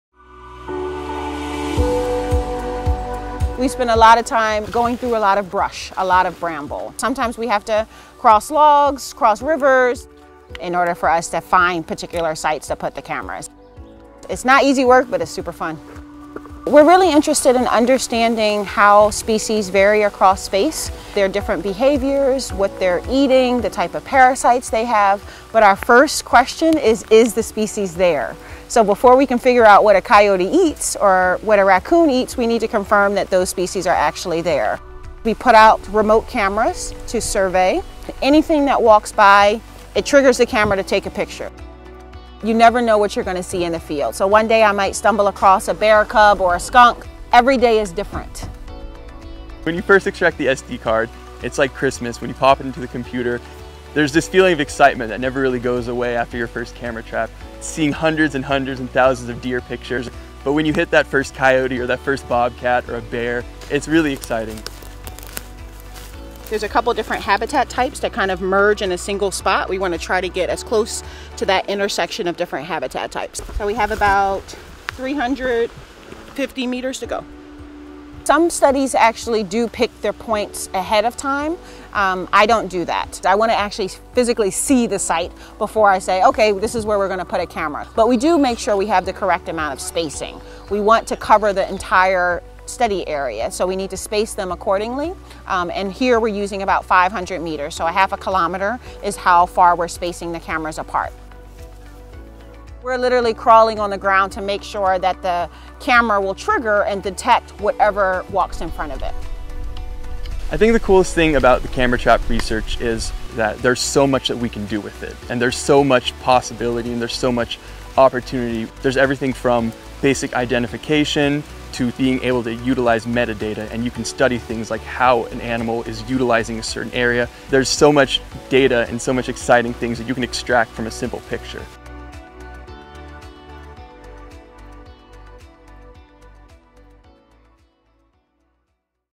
Largest Ever Camera Trap Research of Michigan Wildlife
The U-M Biological Station is one of three Michigan sites where U-M researchers recently installed more than 150 motion-triggered “camera traps” to capture snapshots of the state’s diverse wildlife.
The multi-year wildlife survey will yield hundreds of thousands of snapshots and is the largest-ever camera-trap study of Michigan wildlife. Students from schools across southeastern Michigan will help identify the animals through a new web-based program called Michigan ZoomIN